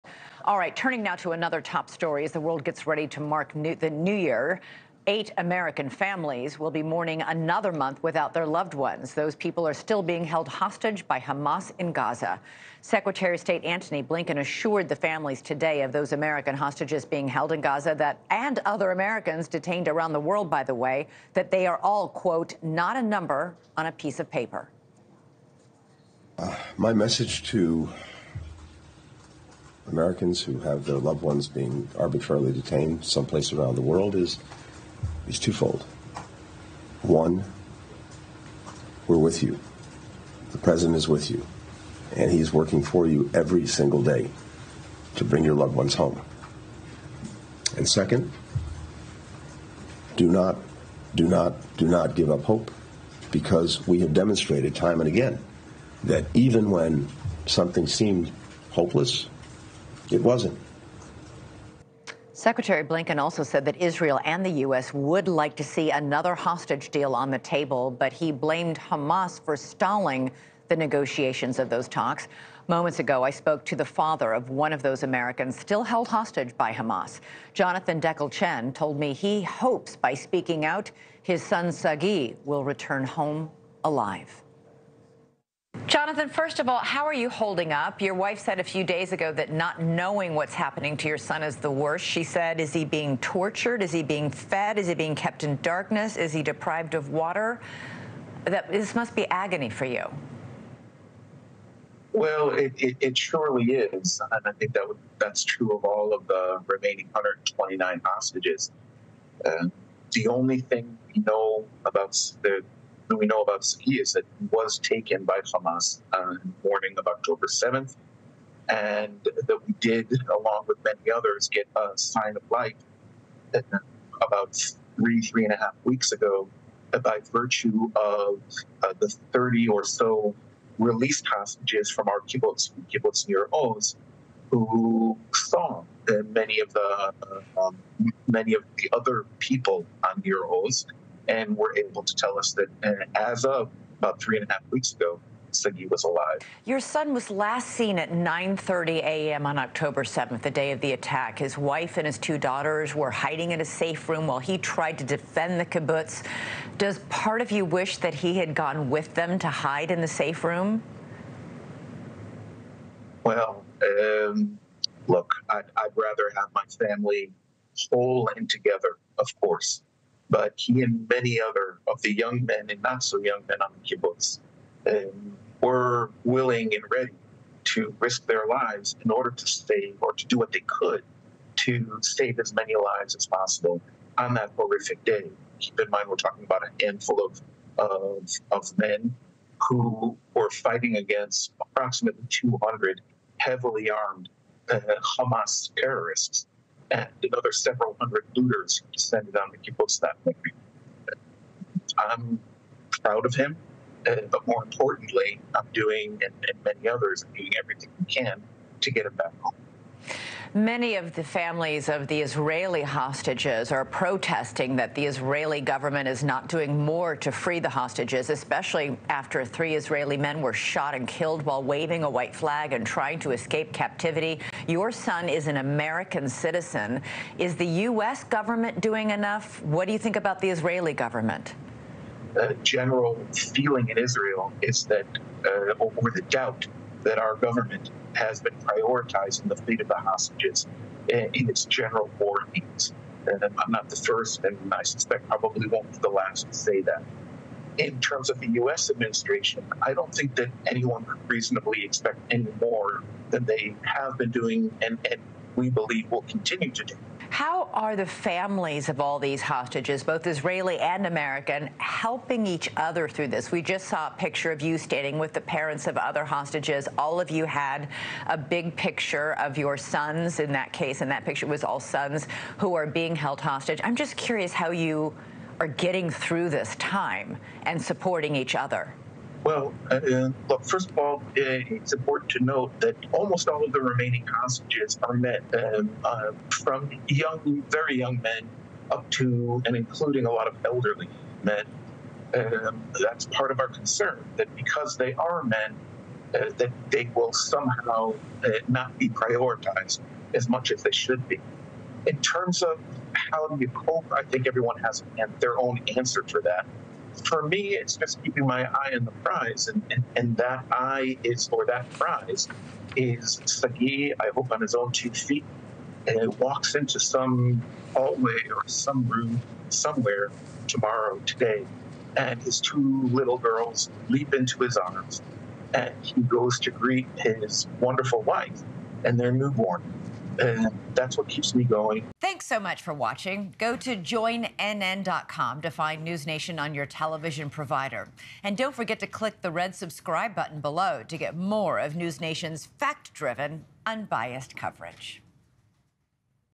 Father of American held hostage by Hamas: He risked life to save others | Vargas Reports
As the world gets ready to mark a new year, eight American families will be mourning another month without their loved ones who are still being held hostage by Hamas in Gaza. Secretary of State Antony Blinken assured the families Wednesday that the hostages — and other Americans detained around the world — are are "not numbers on a piece of paper." Jonathan Dekel-Chen, the father of one of the Americans held hostage by Hamas, hopes by speaking out, his son will return home alive. Dekel-Chen says he's "proud" of his son for fighting to defend the kibbutz from which he was captured.